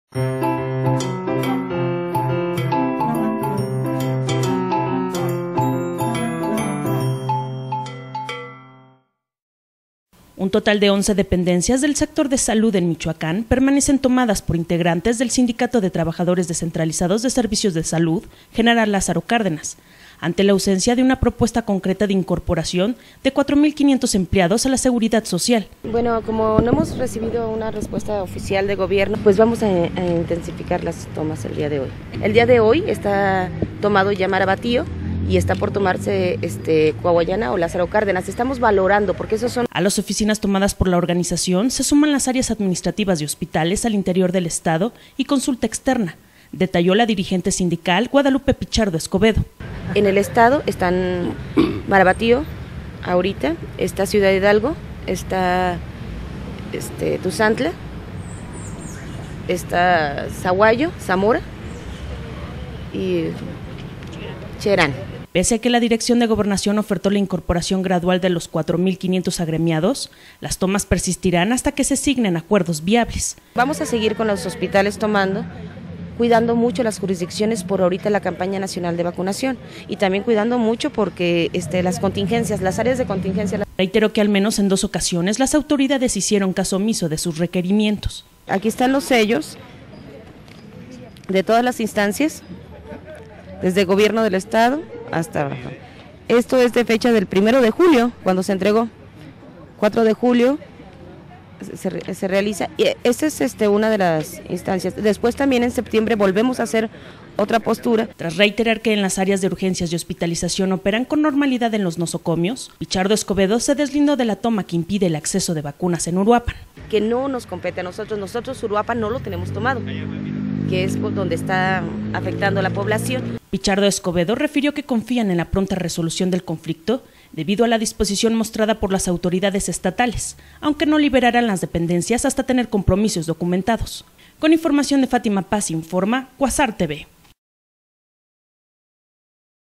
Amenazan con ampliar toma trabajadores de salud
Un total de once dependencias del sector salud en Michoacán permanecen tomadas por integrantes del Sindicato de Trabajadores Descentralizados de Servicios de Salud "Gral. Lázaro Cárdenas", ante la ausencia de una propuesta concreta de incorporación  de cuatro mil 500 empleados a la seguridad...